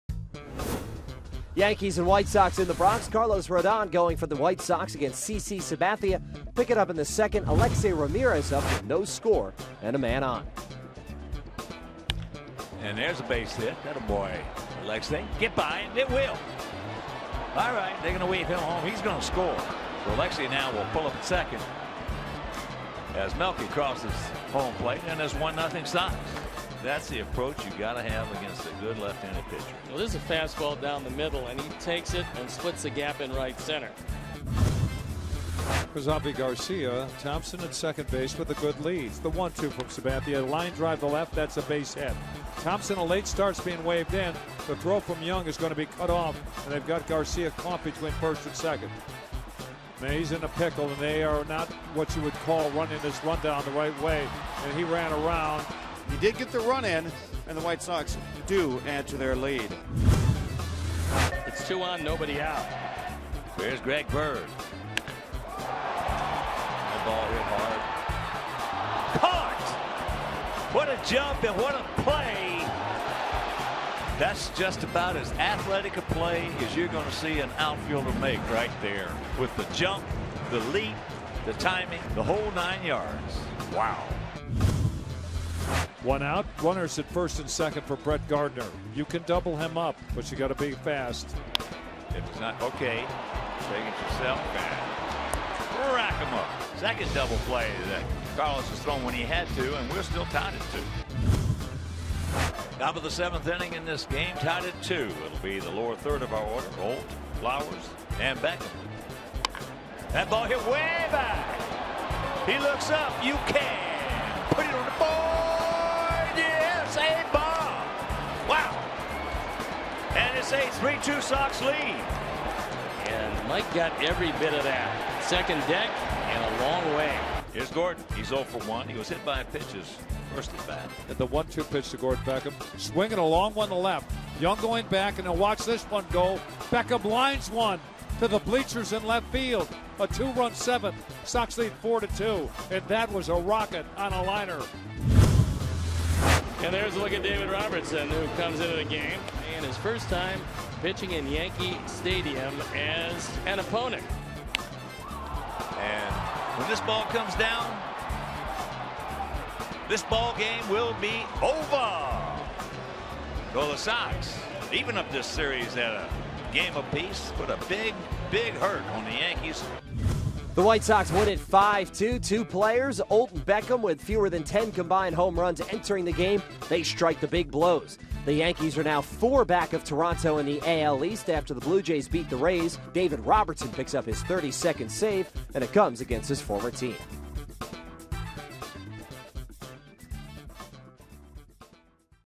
9/25/15: White Sox hold off Yankees to even series
Daily Recap: Mike Olt broke a 2-2 tie in the 7th with a solo shot and David Robertson earned the save in his first outing back in New York